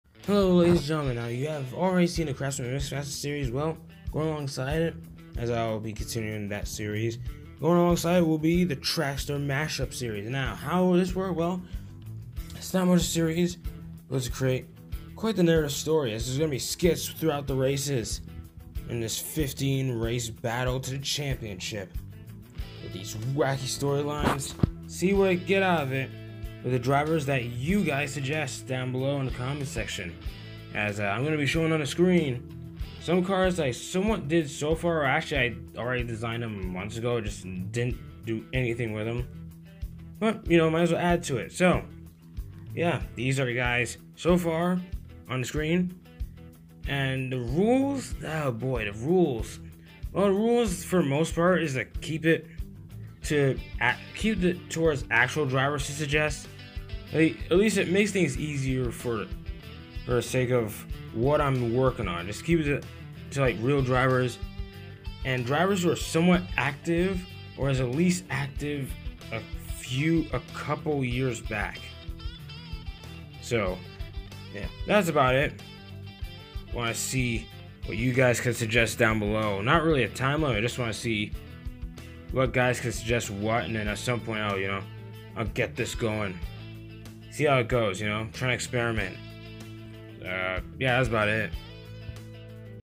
NASCAR Papercraft Driver Suggestions (Comment Below)
Rules:
You could suggest custom drivers, but real drivers are recommended
Try to suggest drivers who haven't been retired for a decade
Whatever cars you see in this video are already part of the series

Not every suggestion is guaranteed to be used

List of confirmed drivers
1. Ryan Blaney #12 Ford, Menards/Libman, Penske (Colin Cropley)
2. Matt Crafton #88 Toyota, Menards/Dutch Boy, Thorsport (Colin Cropley)
3. Austin Dillon #3 Chevy, Bass Pro Shops, RCR (Colin Cropley)
4. Dean Thompson #5 Toyota, Thompson Pipe Group, Tricon
5. Shane Van Gisbergen #97 Chevy, WeatherTech, Kaulig
6. Kaz Grala #15 Ford, N29, Rick Ware
7.
8.
9.
10.
etc.